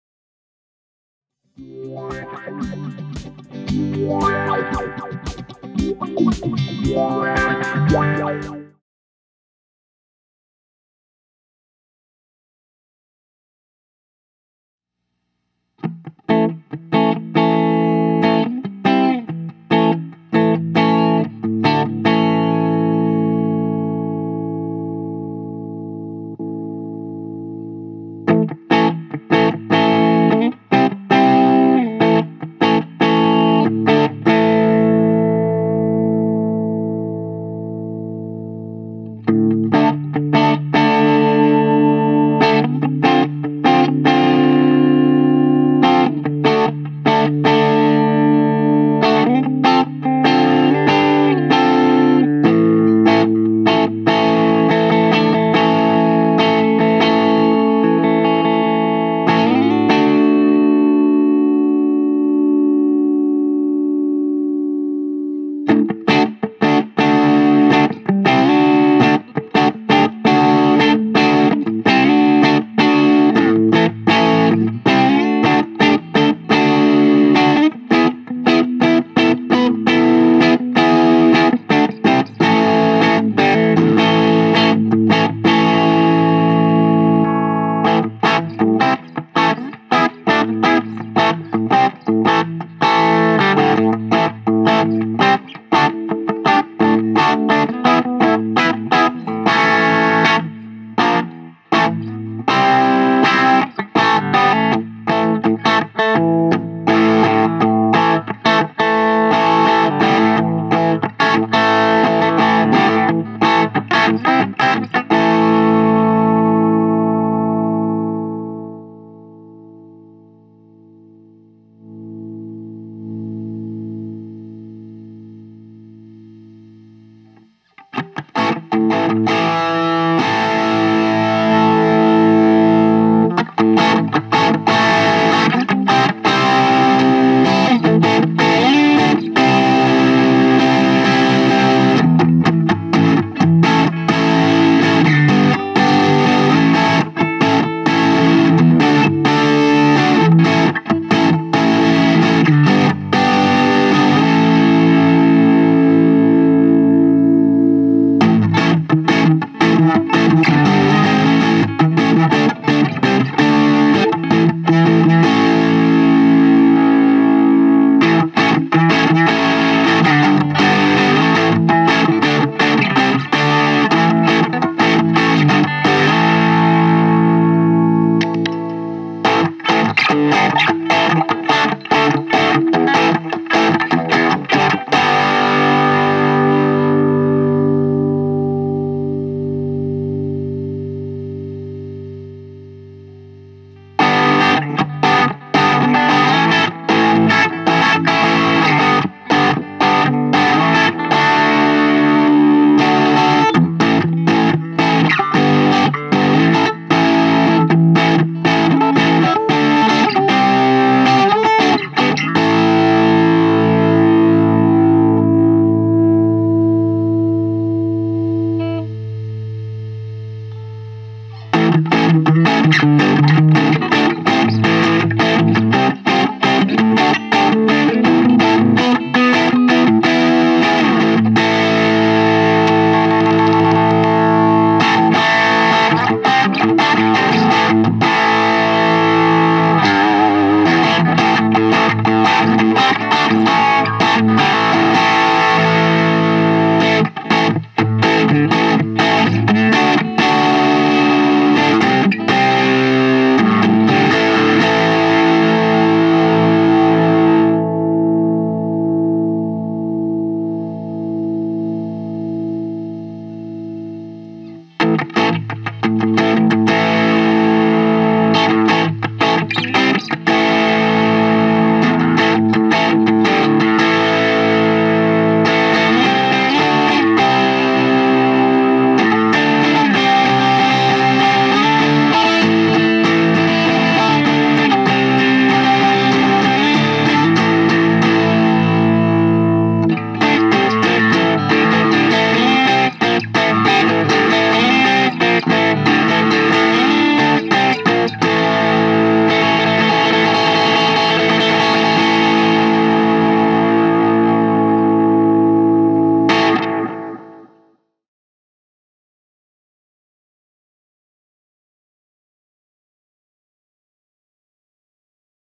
Origin Effects RevivalDRIVE Compact Overdrive
This is a demo video of the RevivalDRIVE Compact Overdrive pedal from Origin Effects performed and produced by Pedal of the Day. for more effects pedal reviews and demos, and be sure to check out more of the videos on our YouTube channel as well. Cheers!

Spectraflex Cables